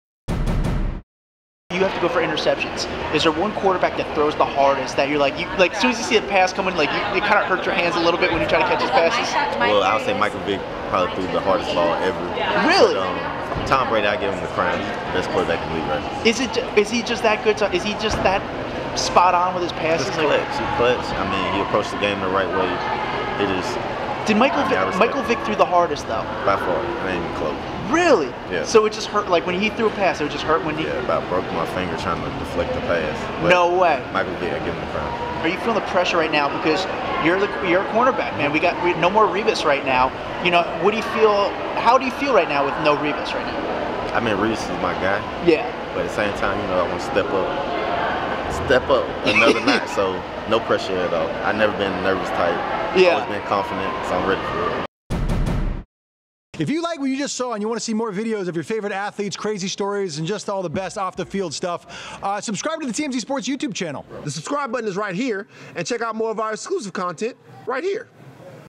BUSTER SKRINE MICHAEL VICK NEARLY BROKE MY FINGER 'Hardest-Throwing QB, Not Even Close' | TMZ Sports
N.Y. Jets stud Buster Skrine says Michael Vick is without a doubt the hardest-throwing QB he's ever faced ... telling TMZ Sports he almost broke his finger deflecting one of Vick's passes.